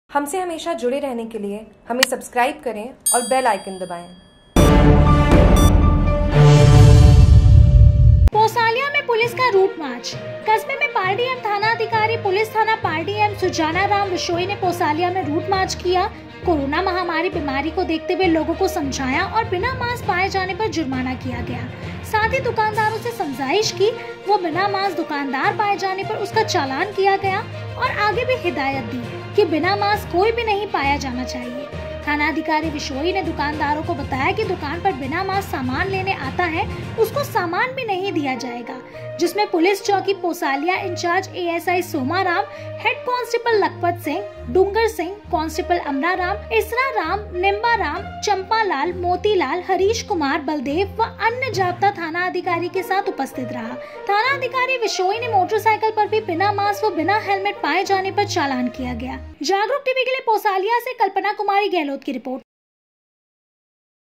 Jagruk TV : पोसालिया में पुलिस का रूट मार्च, बिना मास्क पाए जाने पर जुर्माना
कस्बे में पालड़ी एम थानाधिकारी पुलिस थाना पालड़ी एम सुजाना राम बिश्नोई ने पोसालिया में रूट मार्च किया। कोरोना महामारी बीमारी को देखते हुए लोगों को समझाया और बिना मास्क पाए जाने पर जुर्माना किया गया। साथ ही दुकानदारों से समझाइश की व बिना मास्क दुकानदार पाए जाने पर उसका चालान किया गया और आगे भी हिदायत दी कि बिना मास्क कोई भी नहीं पाया जाना चाहिए। थानाधिकारी विश्नोई ने दुकानदारों को बताया कि दुकान पर बिना मास्क सामान लेने आता है, उसको सामान भी नहीं दिया जाएगा। जिसमें पुलिस चौकी पोसालिया इंचार्ज Asi सोमाराम, हेड कांस्टेबल लखपत सिंह, डूंगर सिंह, कांस्टेबल अमराराम, इसरा राम, निंबाराम, चम्पा लाल, मोतीलाल, हरीश कुमार बलदेव व अन्य जाब्ता थाना अधिकारी के साथ उपस्थित रहा। थानाधिकारी विश्नोई ने मोटरसाइकिल पर भी बिना मास्क वह बिना हेलमेट पाए जाने पर चालान किया गया।

 जागरूक टीवी के लिए पोसालिया से कल्पना कुमारी गहलोत की रिपोर्ट

Jagruk TV